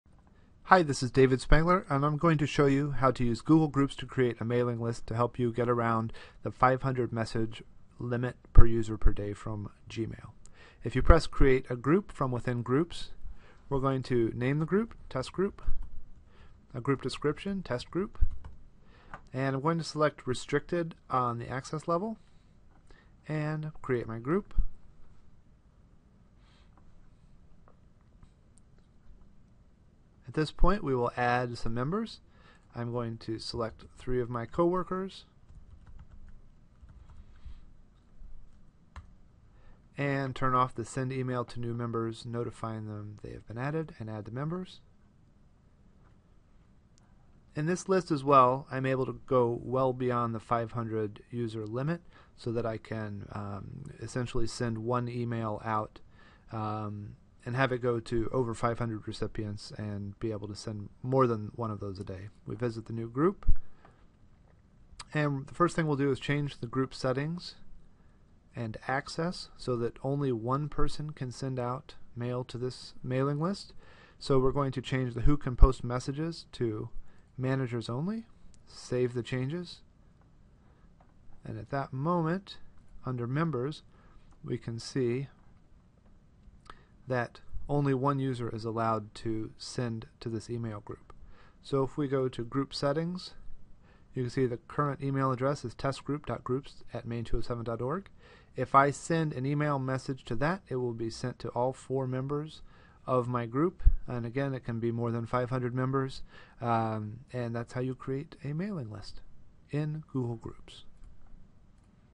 How To Use Google Groups as a Mailing List
How To Use Google Groups as a Mailing List, helping to get around the 500 message per day limit on a Gmail account.